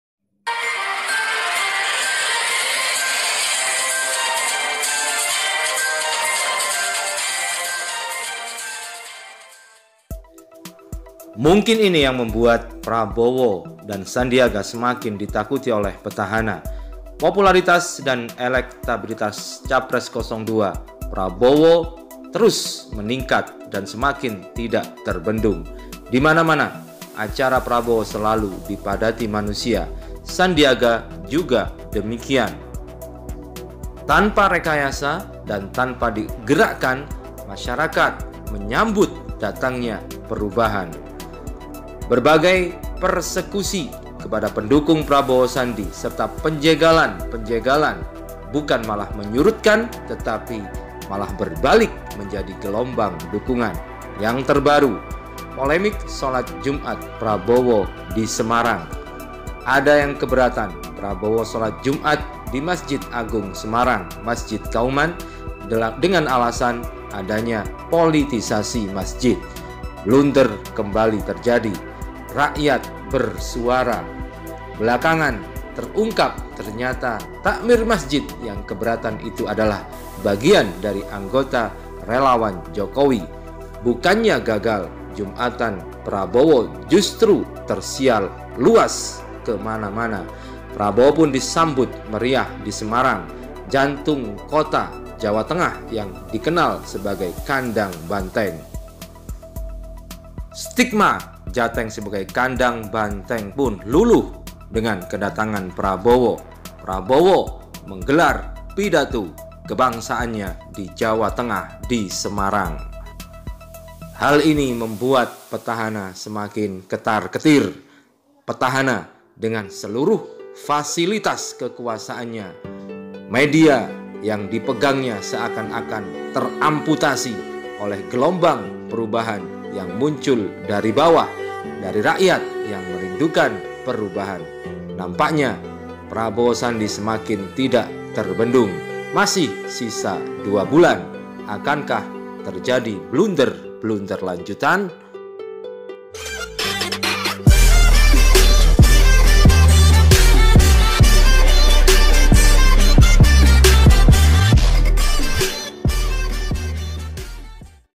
INIKAH YG MEMBUAT PRABOWO DITAKUTI PETAHANA?;GELOMBANG PERUBAHAN DARI BAWAH;PILPRES 2019;JOKOWI;
17 FEBRUARI 2019
PRABOWO-SANDI;JOKOWI-MA'RUF AMIEN